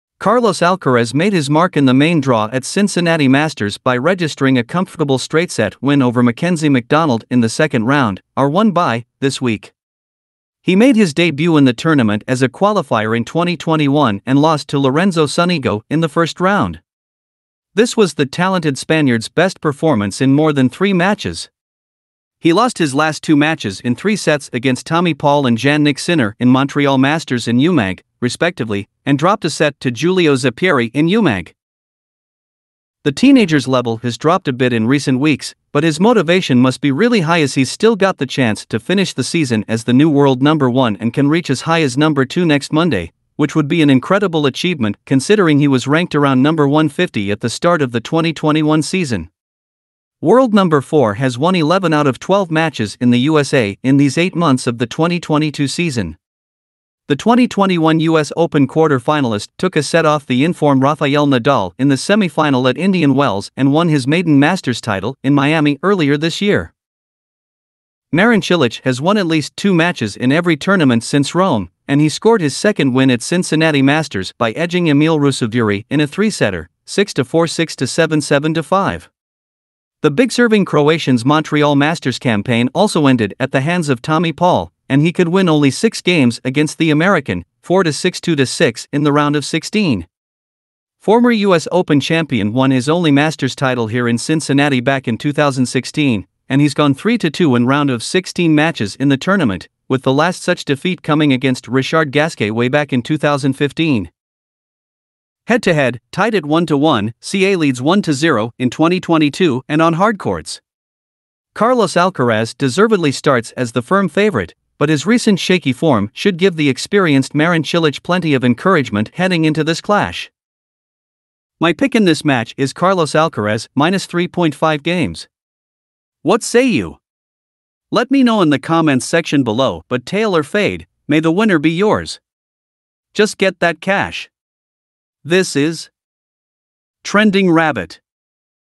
Carlos Alcaraz vs Marin Cilic ATP Cincinnati Tennis Pick and Prediction 8/18
Carlos Alcaraz takes on Marin Cilic in Thursday's Tennis Cincinnati Action. Get Trending Rabbit's Tennis Cincinnati Pick and Prediction for this game.

Join the Trending Rabbit family on telegram where we chat sports gambling and live in-play betting 24/7 everyone is welcome